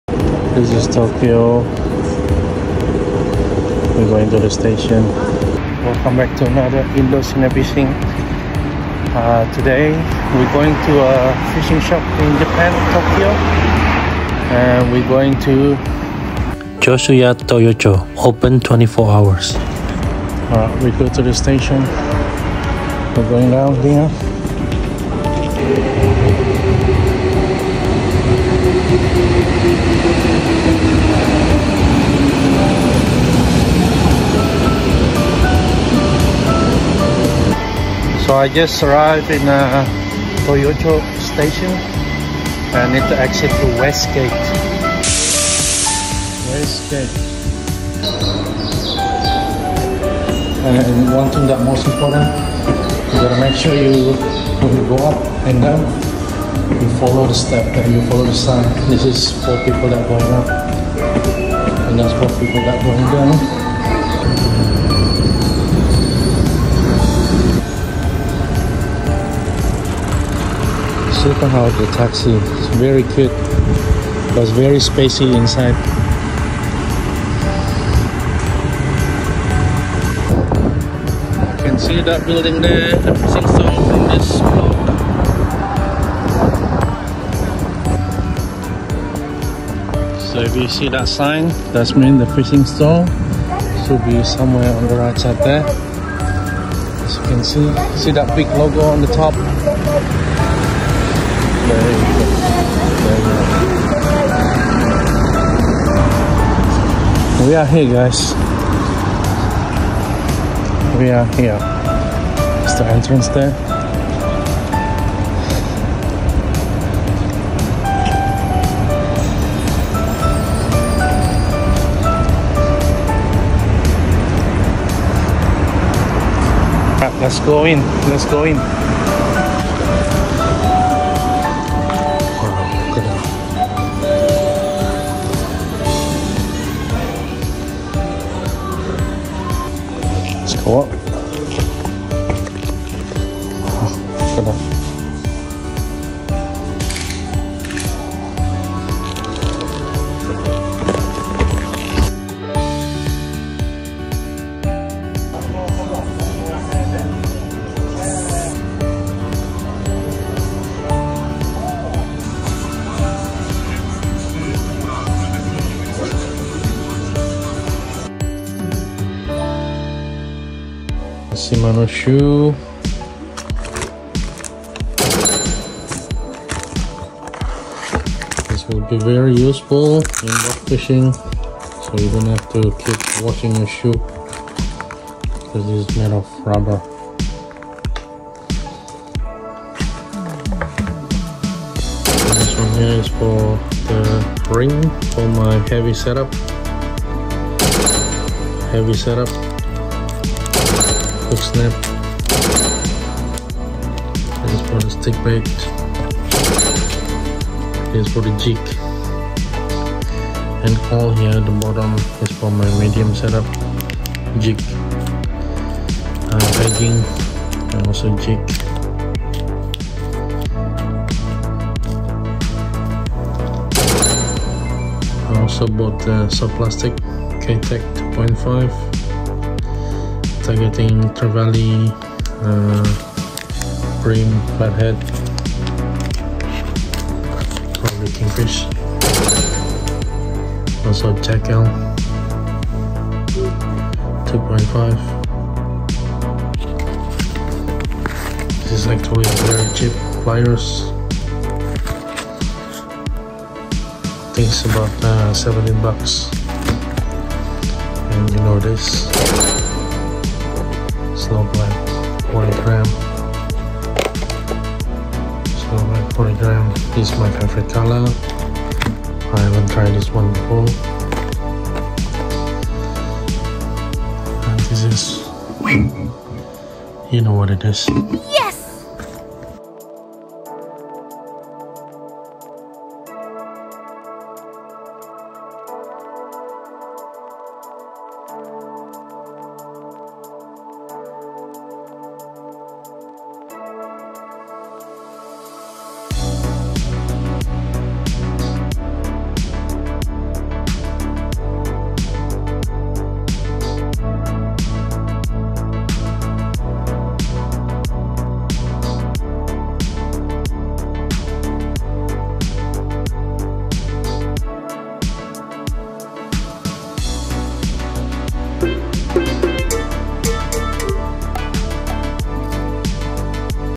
Shopping fishing gears Tokyo Japan
A little vlog about how I get to the fishing store in Tokyo, Japan.

Unfortunately the store manager not allowing full videos which I totally respected. However I was only captured few pictures and short clips.

The store was huge and open 24 hours!

Give them a visit when you are in Tokyo.

Joshuya Toyocho つり具の上州屋 東陽町店

7 Chome-5-4 Toyo, Koto City, Tokyo 135-0016, Japan

〒135-0016 東京都江東区東陽７丁目５−4